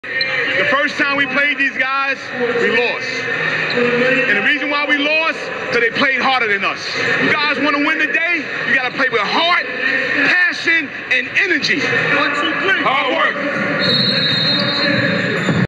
Inspirational Speech
ESPN/BallUp Challenge
Coach Tone gives players an inspirational speech
just minutes before game time.